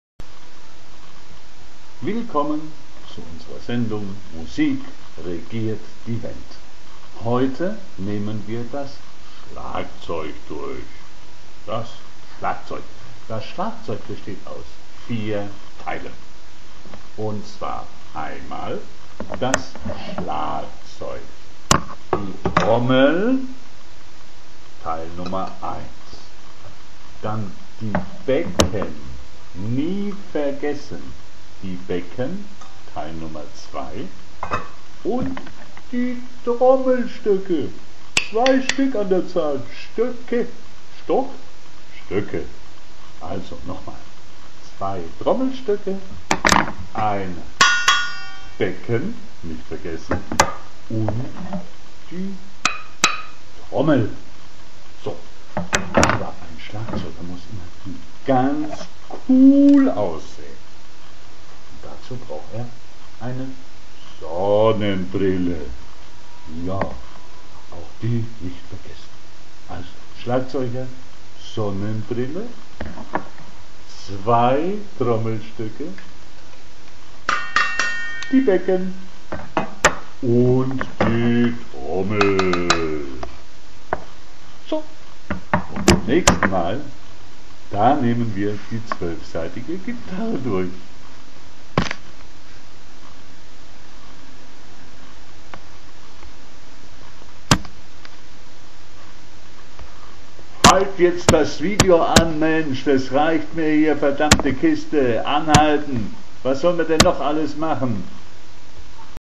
Das Schlagzeug
Genaue Erklärung meines Schlagzeugs